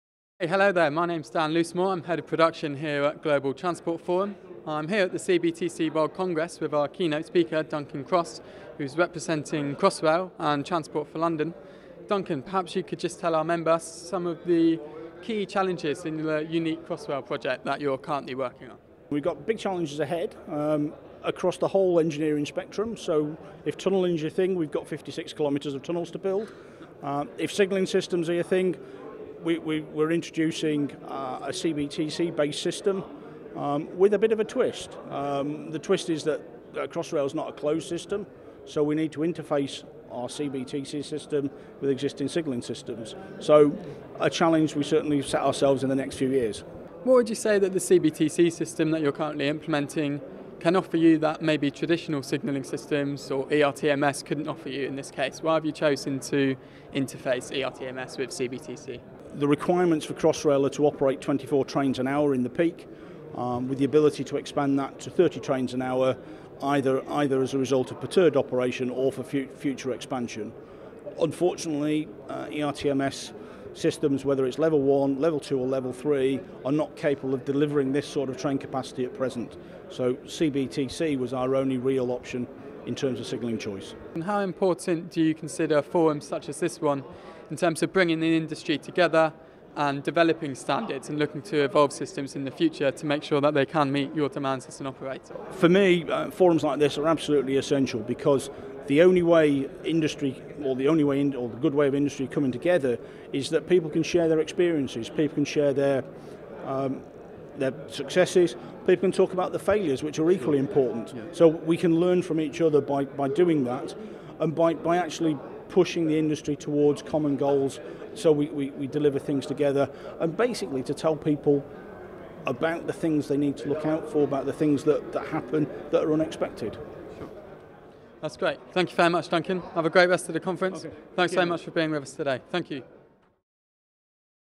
Duncan Cross from Crossrail at the CBTC World Congress
Daniel Loosemore, Head of Production at Global Transport Forum, speaks to Duncan Cross (Crossrail) at the CBTC World Congress on the importance of CBTC and the business case for the Crossrail project. for further details